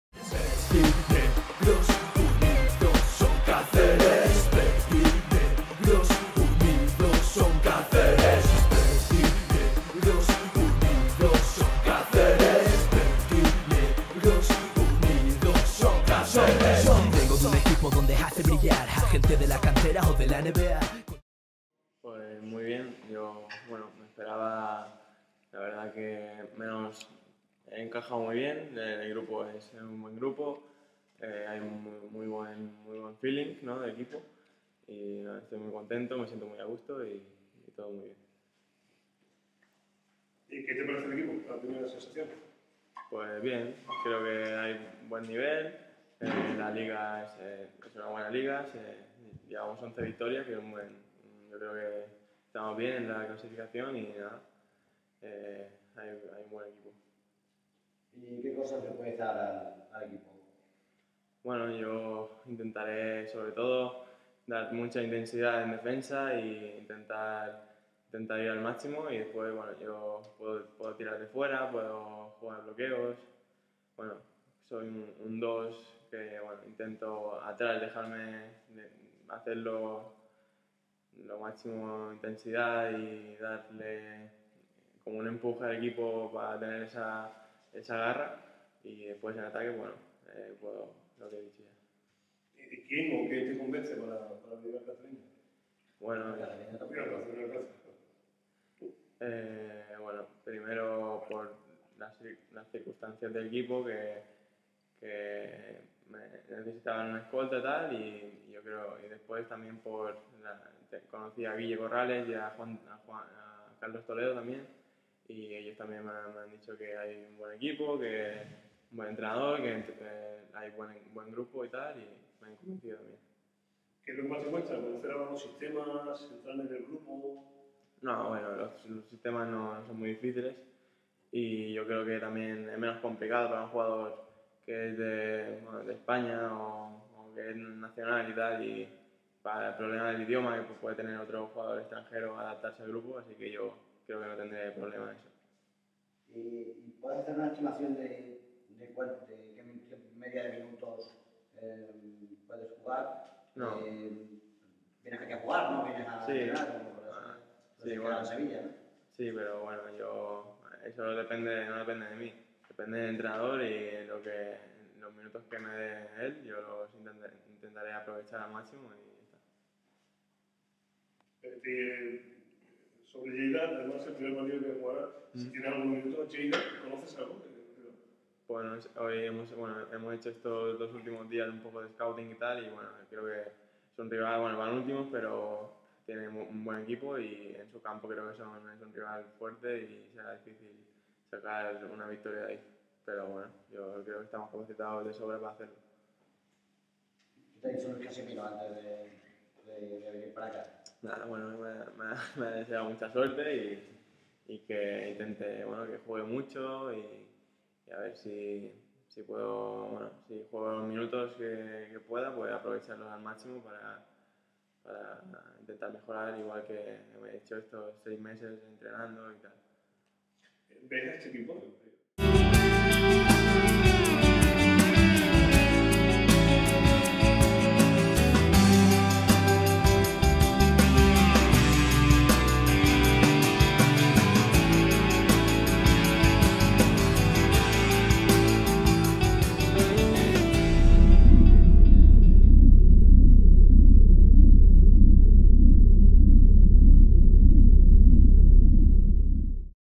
t1516pj24 tomeurigo lleida
Caceresbasket 2015/16 La última incorporación del Cáceres Patrimonio de la Humanidad, Tomeu Rigo, comparece ante los medios de comuniación en la previa de la jornada 24 de la liga LEB Oro antes de viajar a Lleida para enfrentarse al Actel Força en el que será su debut como jugador verdiengro